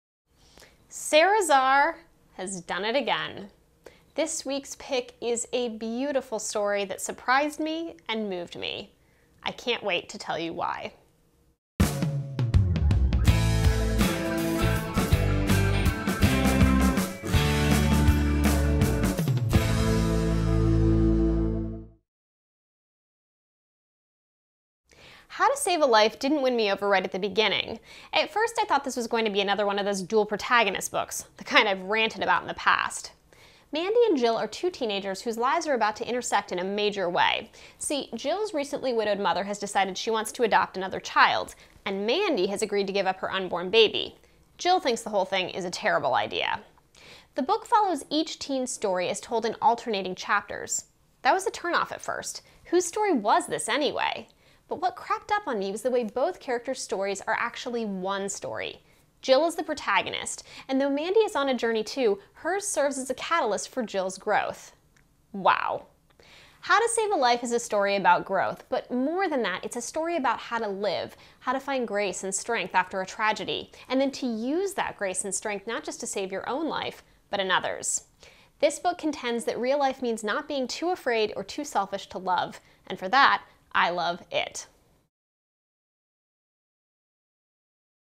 "How to Save a Life" | 60second Book Review
"How to Save a Life" by Sara Zarr. Books for Teens: 60second Book Review by Jenny Sawyer.
As though Jill MacSweeny's life wasn't horrible enough after the death of her father, her mother has just broken the news that Project: Replace Dad is well underway. At least, that's how Jill sees it. Her mom's plan? Adopt a baby from Mandy, a teenage mother. But it seems to Jill as though Mandy's baby isn't the only one looking for a home.

As Mandy inveigles her way into the MacSweenys' lives—and, eventually, their hearts—Jill discovers that there may just be enough grace left in this world to save all of them.